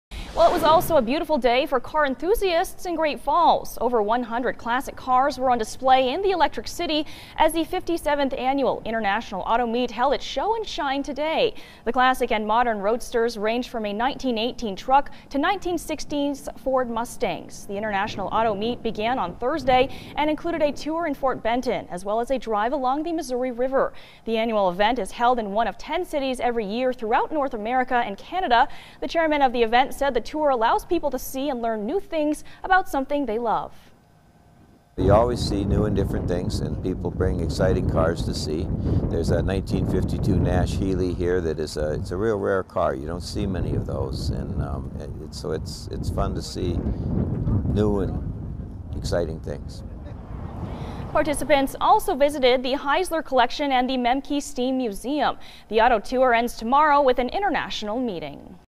International Auto Meet highlights classic cars across U S and Canada
108 cars were on display in Great Falls on Saturday for the Show and Shine Car Show